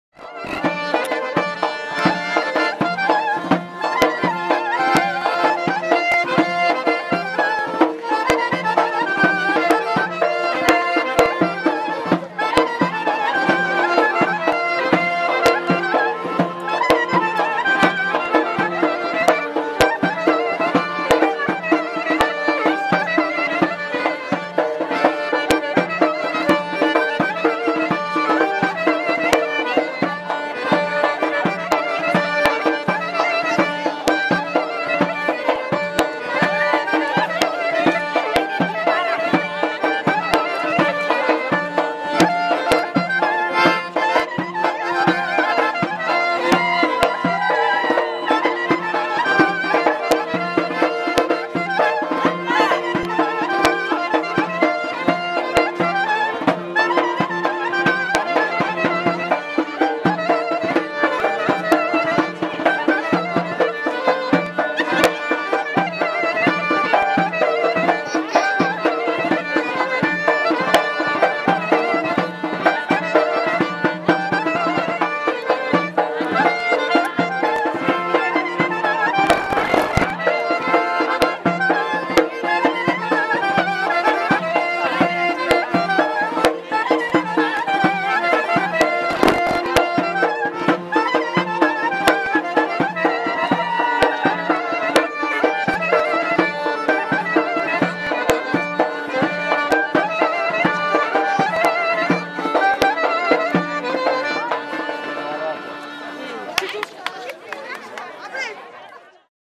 Շուրջպար (գ. Նորշեն-Ծղալթբիլա)
Հատված Թելմանի և Նարինեի հարսանիքից, 2012 թ.